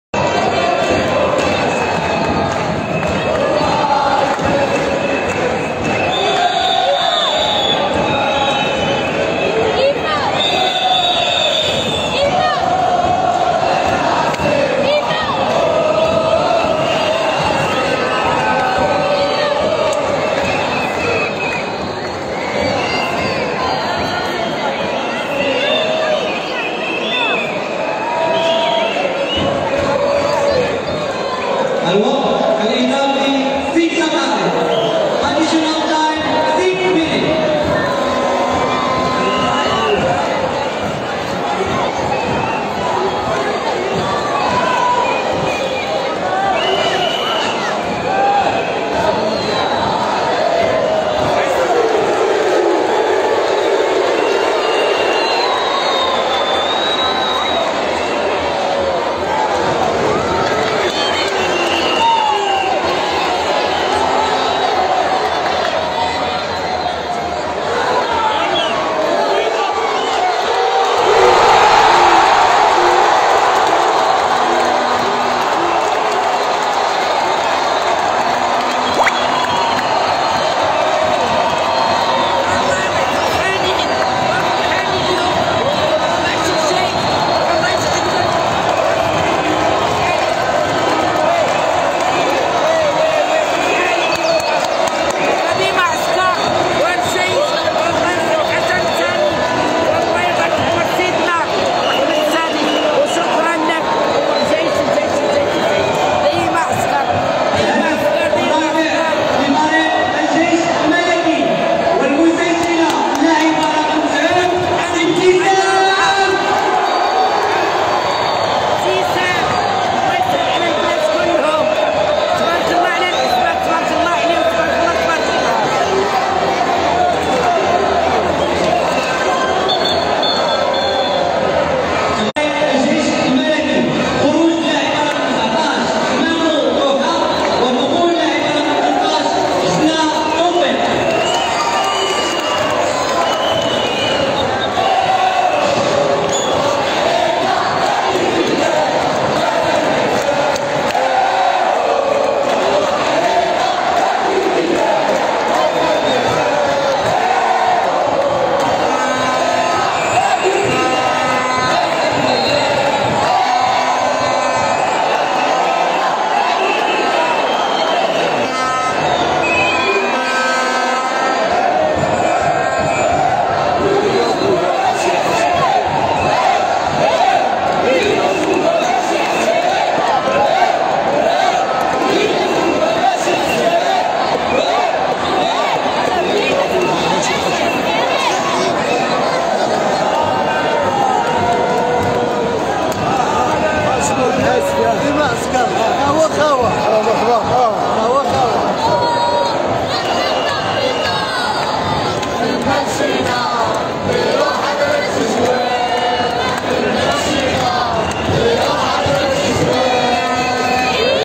فرحة جنونية فالمدرجات لجماهير الجيش الملكي بعد تسجيل الهدف الثالث في شباك ماميلودي صن داونز
مباشرة من الرباط.. فرحة جنونية فالمدرجات لجماهير الجيش الملكي بعد تسجيل الهدف الثالث في شباك ماميلودي صن داونزفي نهائي عصبة الأبطال الأفريقية لسيدات